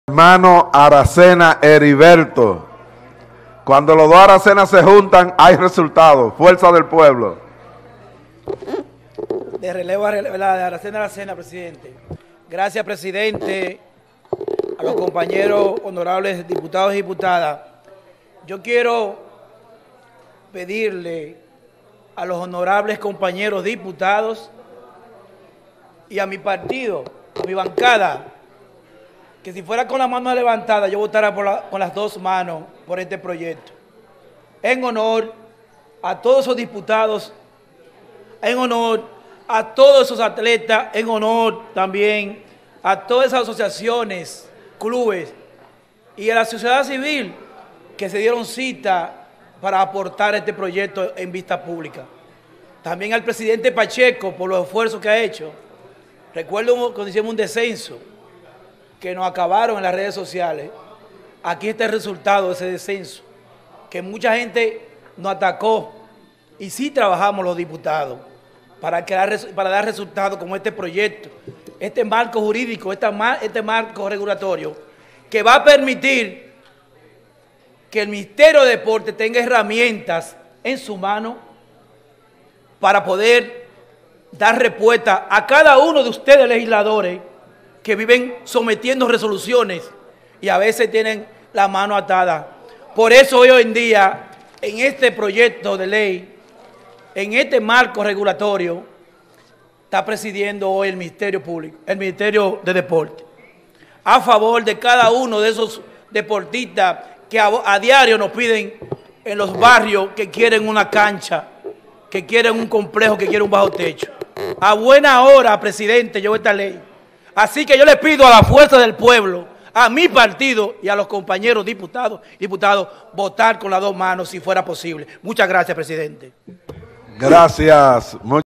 Turno del diputado Heriberto Aracena, Sesión Ordinaria No. 00038 jueves 12 de enero 2023.
El diputado @heriberto0316 solicita el voto favorable a este proyecto en honor a todos los deportistas y a los que serán beneficiados con la aprobación de este Marco Normativo regulatorio para la Ley No. 356-05.